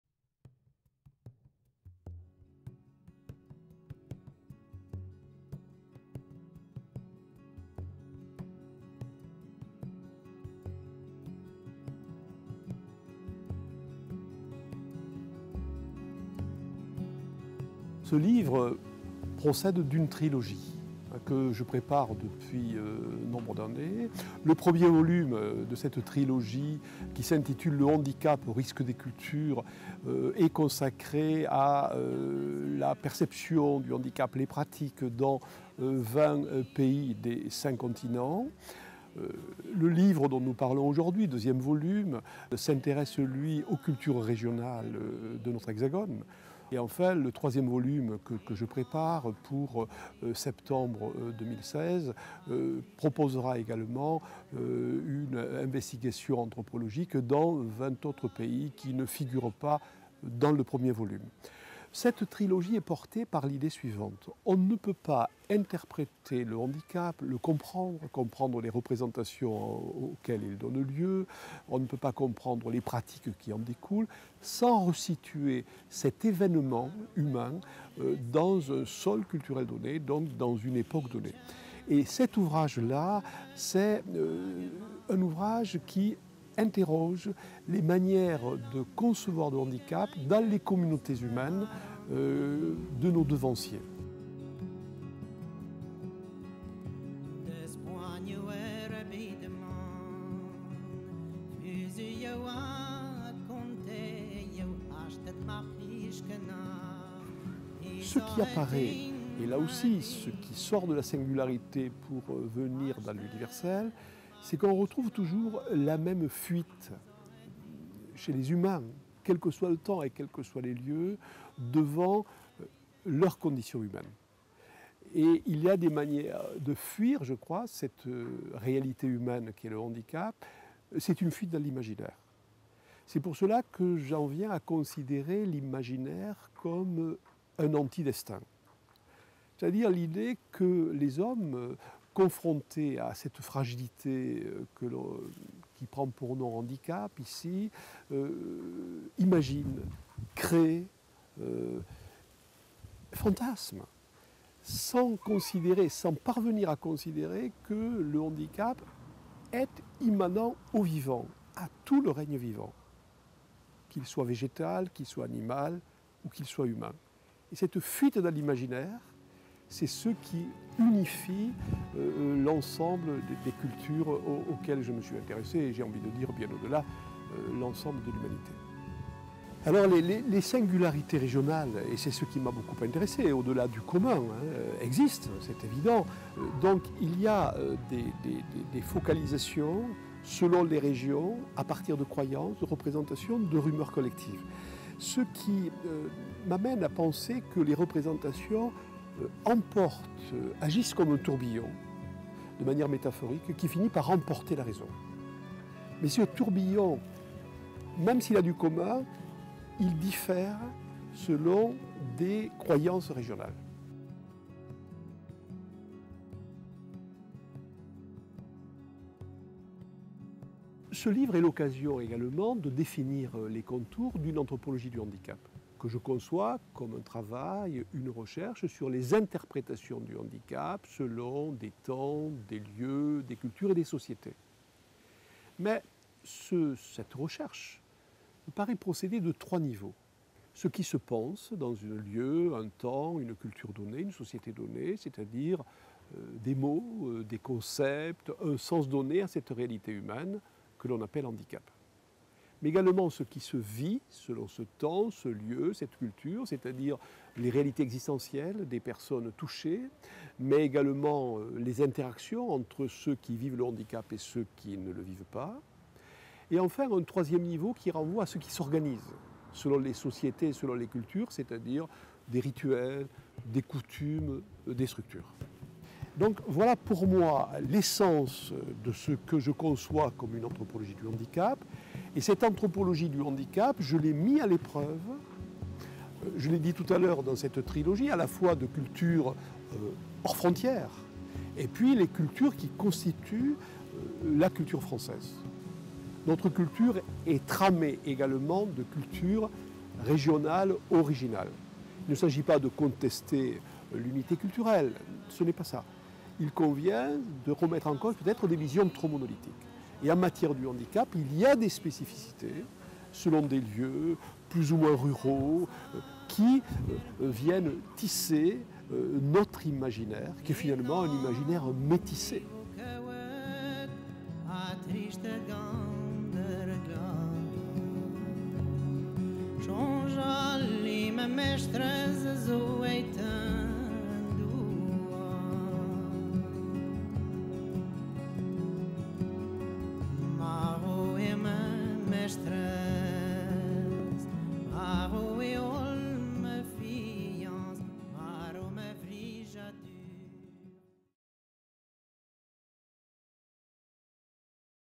GARDOU Charles : Handicap dans notre imaginaire culturel
Cet ouvrage propose un périple anthropologique inédit à l’intérieur même de nos frontières. Du sud au nord de l’Hexagone,  il offre un florilège d’expressions culturelles autour du handicap. En Quercy,  Forez, Gascogne, Aveyron, Poitou, Corse et dans les Alpes, en Bretagne, Beauce, pays d’Othe et Alsace, quelles significations les communautés humaines attribuaient-elles au handicap ? Quelles illusions collectives partageaient-elles face à la blessure, aux « irrégularités » du corps ou de l’esprit ?

La plongée au cœur de ces « échantillons de civilisations » découvre les profondes racines de notre patrimoine imaginaire, avec son cortège de représentations immémoriales, dont nous discernons, aujourd’hui encore, des empreintes multiples et enchevêtrées.

Ces trésors anthropologiques inédits, dénichés dans des cultures populaires d’antan, montrent que les représentations du handicap agissent comme un tourbillon qui, de tout temps, emporte la raison.